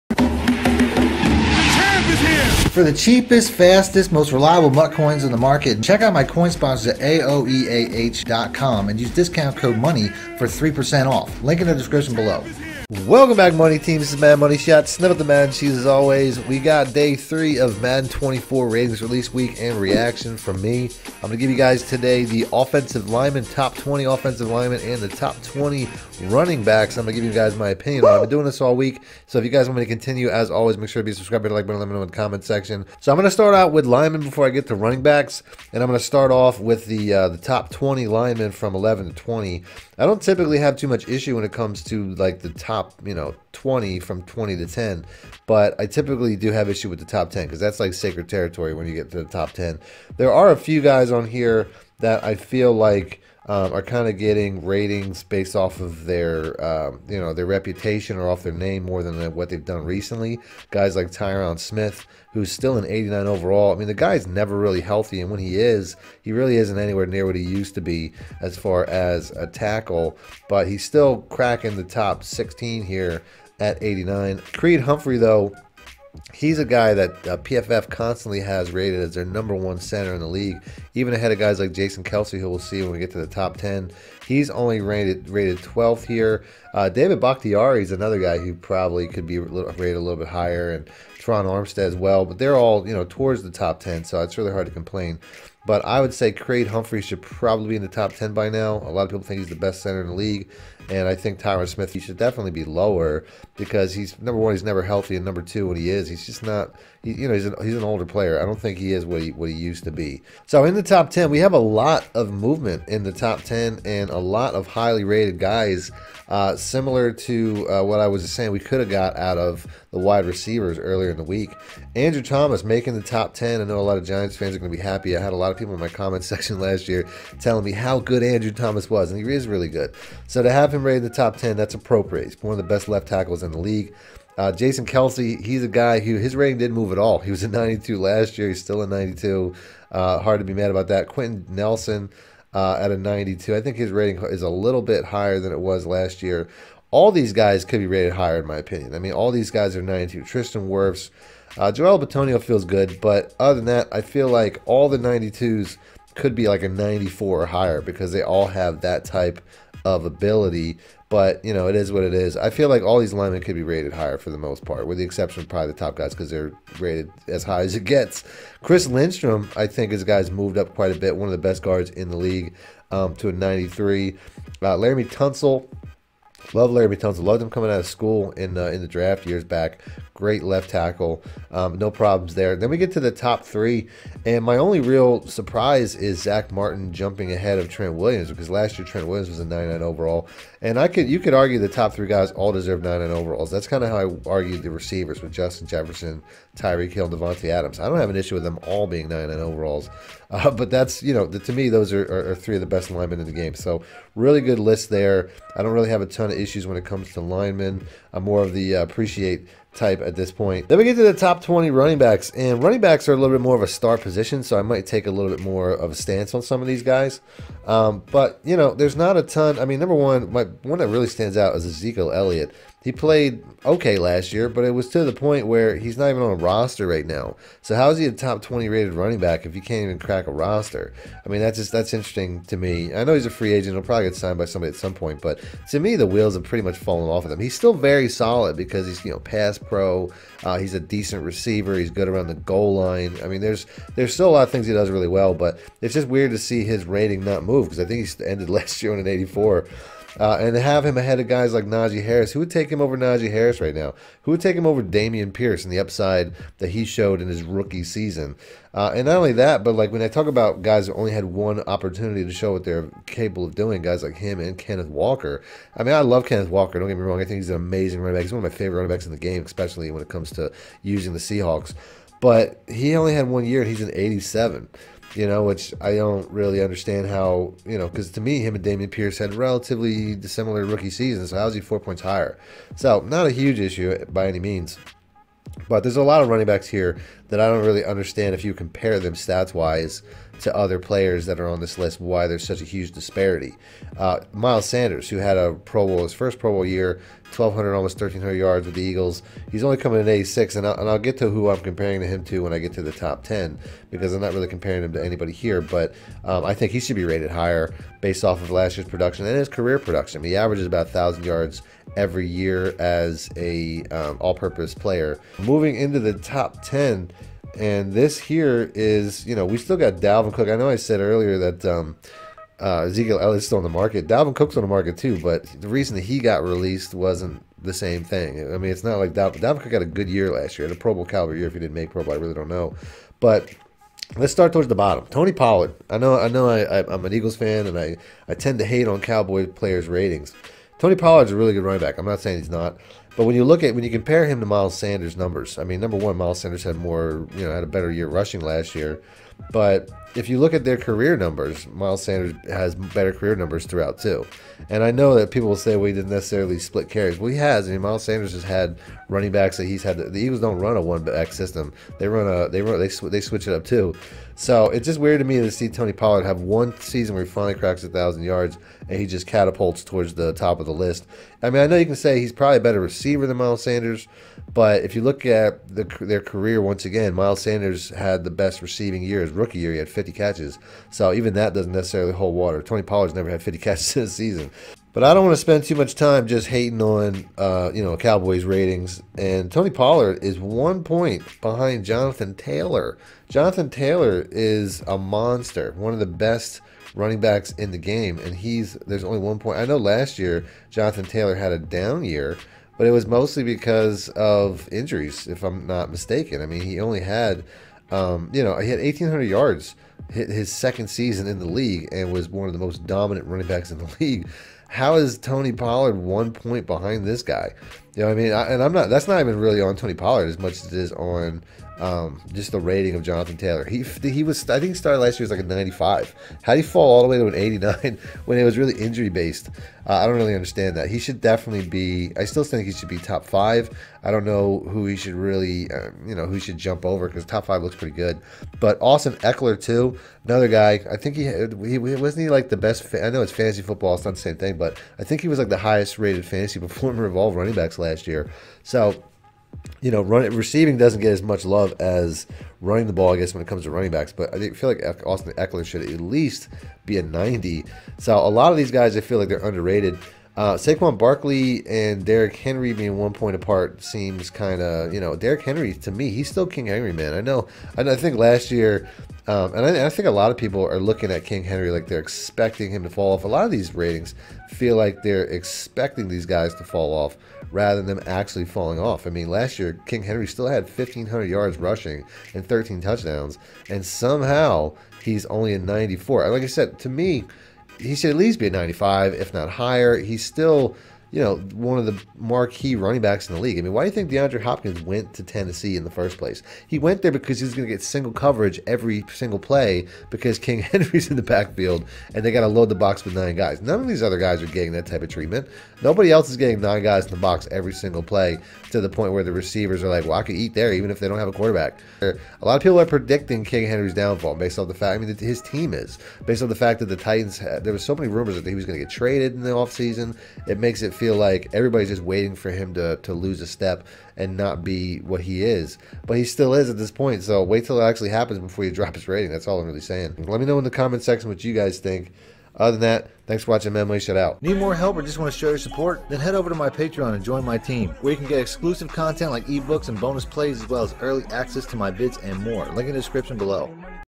Top 10 Running Back & Offensive Lineman! Official Madden NFL 24 Player Ratings Reaction
in this video I will go over the top 10 offensive lineman and running backs in madden nfl 24 player ratings release and give you my reaction to the rankings

BEST OFFENSE PLAYBOOKS

BEST DEFENSE PLAYBOOKS

0:00 Madden NFL 24 Player Ratings
0:33 Offensive Lineman
3:30 Running Backs

NORTH

SOUTH

FALCONS

BUCS

PANTHERS

WEST

SEAHAWKS

CARDINALS

RAMS

AFC

EAST

NORTH

STEELERS

WEST